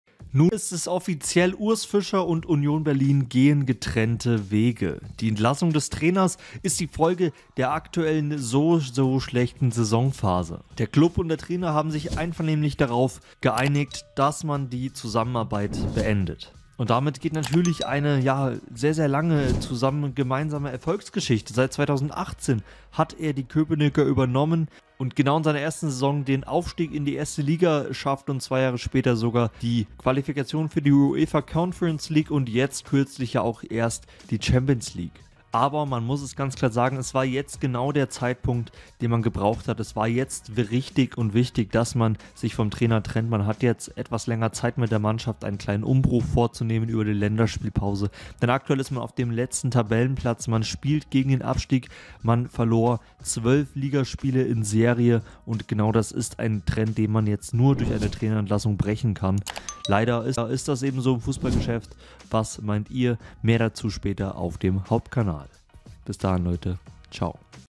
Offiziell: Union Berlin entlässt Trainer Urs Fischer!
Urs Fischer verlässt Union Berlin. Genau das wurde gerade eben durch die Offiziellen des Klubs bestätigt! Eure Meinung?

Photo-Credits: IMAGO
Video-Credits: SID